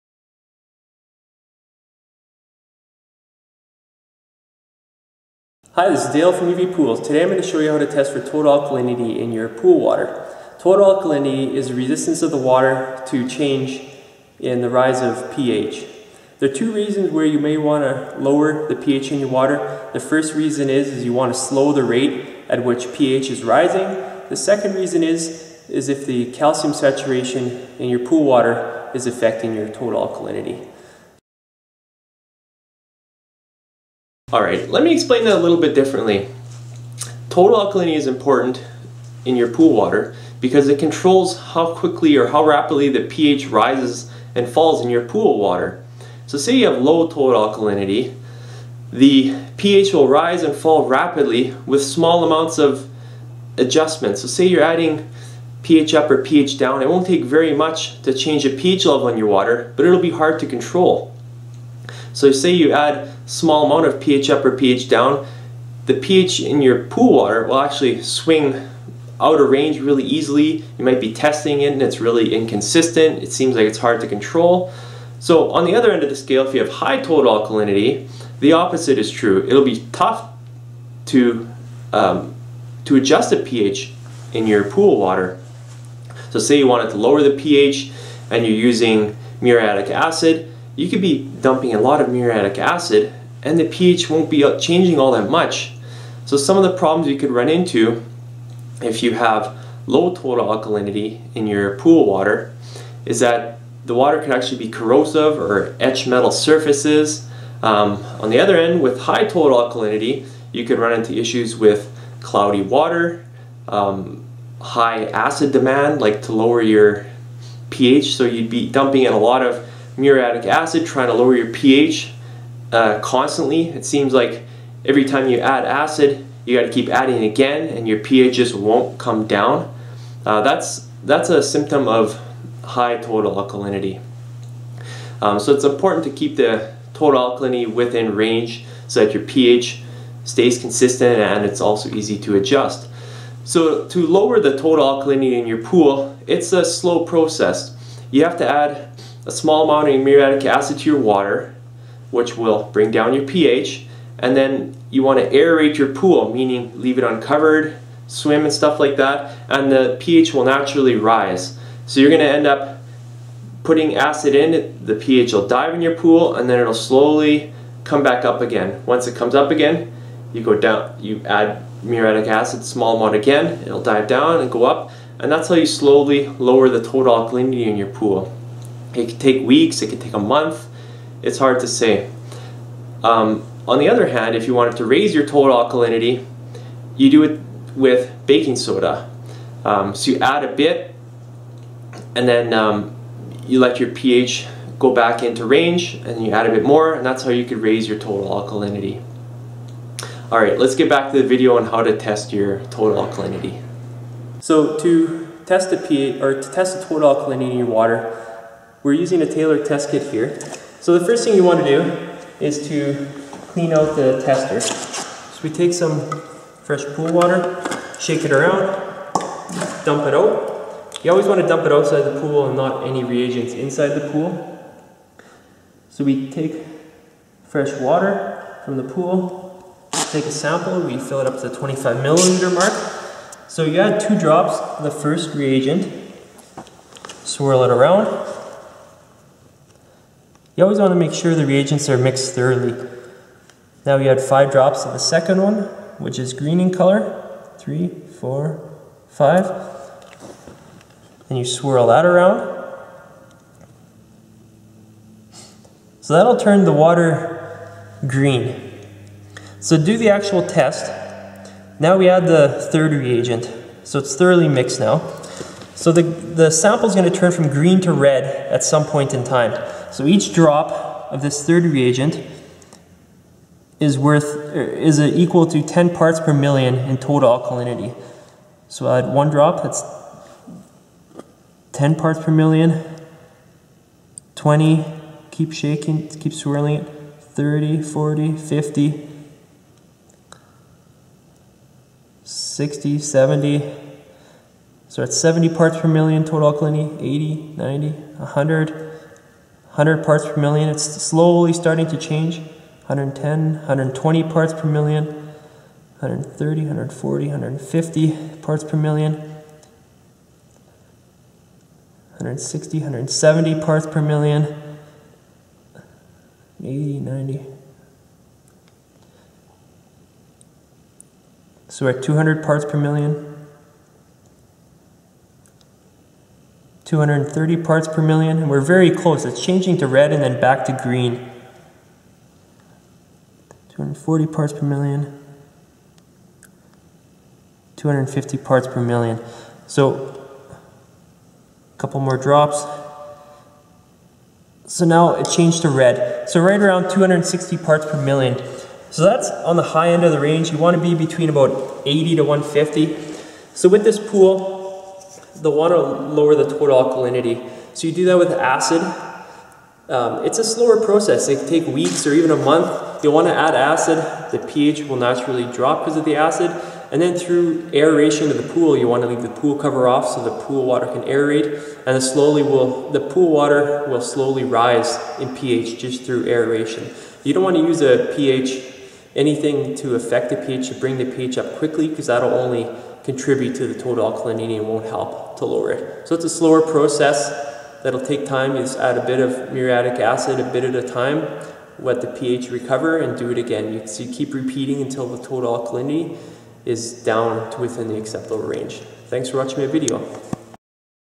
Testing Total Alkalinity in Your Swimming Pool
Instructions on Testing Total Alkalinity in Your Swimming Pool. Total alkalinity is important in your pool because it controls how quickly the pH rises and falls in your pool water. So if you have low total alkalinity the pH will rise and fall rapidly with small amounts of adjustments. Say you're adding pH down it won't take very much to change a pH level on your water but it will be hard to control. If you add small amount of pH up or pH down the pH in your pool will swing out of range really easily. You might be testing it and its really inconsistent and seems like it's hard to control. On the other end of the scale if you have high total alkalinity the opposite is true. It'll be tough to to adjust the pH in your pool water. Say you want to lower the pH and you're using your muriatic acid you could be dumping a lot of muriatic acid and the pH wont be changing all that much. There are many problems you can run into if you have low total alkalinity in your pool water. The water can actually be corrosive or etch metal surfaces, on the other end with high total alkalinity you could run into issues with cloudy water high-acid demand, to lower your pH you'd be dumping a lot of muriatic acid to lower your ph constantly. It seems like every time you add acid you have to keep adding again and your pH won't come down that's that's a symptom of high total alkalinity. This is why it's important to keep the total alkalinity within range so your pH stays consistent and it's also easy to adjust. so to lower the total alkalinity your pool it's a slow process you have to add a small amounts of muriatic acid into your water which will bring down your pH and then you want to aerate your pool meaning leave it uncovered. You can still swim and stuff like that and the pH will naturally rise so you're going to end up putting acid in the pH will dive in your pool and then slowly come back up again. Once it comes up again you add muriatic acid small one again it'll die down and go up. That's how you slowly lower the total alkalinity of your pool. It can take weeks it could take a month it's hard to say. On the other hand if you want to raise your total alkalinity you do it with baking soda, add a bit and then you let your pH go back into range and you add a bit more that's how you raise your total alkalinity.

UV Pools
4304 Henderson Hwy Narol, MB R1C 0A1

We are located just outside the Winnipeg city limits.